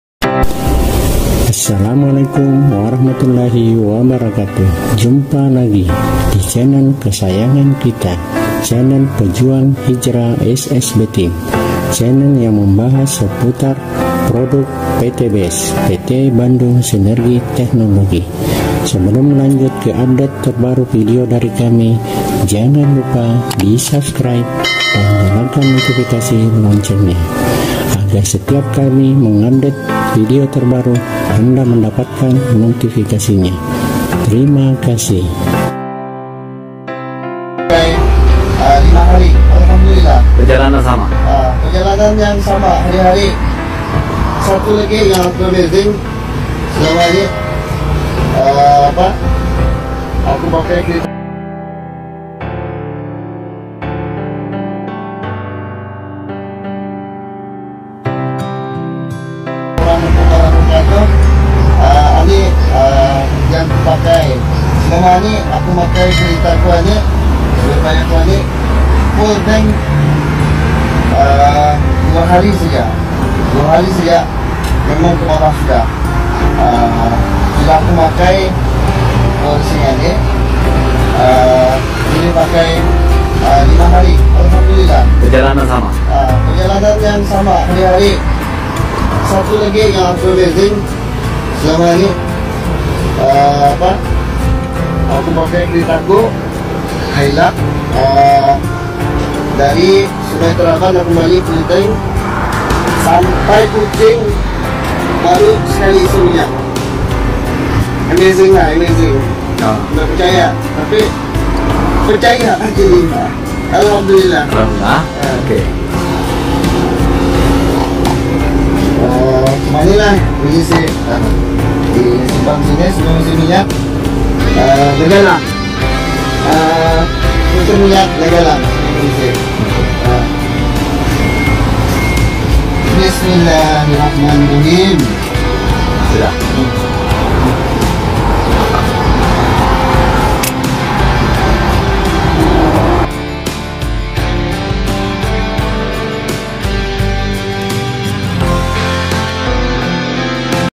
Eco Racing Di Malaysia..Biasanya Ngisi BBM Full Tank Pakenya 2 Hari Setelah Pake Eco Racing 5 hari
Luar Biasa Iritnya Testimoni Eco Racing Dari Negri Jiran, Biasanya Full Tank Pakenya 2 Hari Setelah Menggunakan Eco Racing Menjadi 5 hari Dengan Rute/Perjalanan Yang Sama Setiap Hari..!!

Masih Ragu..?

🚨MANFAAT ECO RACING :
♨️Meningkatkan 5 - 10 RON
♨️Menghilangkan Knocking/Mrebet
♨️Menambah tenaga dan akselerasi
♨️Menghemat bbm hingga 50%
♨️Membersihkan Kerak
♨️Melindungi mesin dengan Nano Teknologi
♨️Menghilangkan emisi CO (polusi) hingga 100%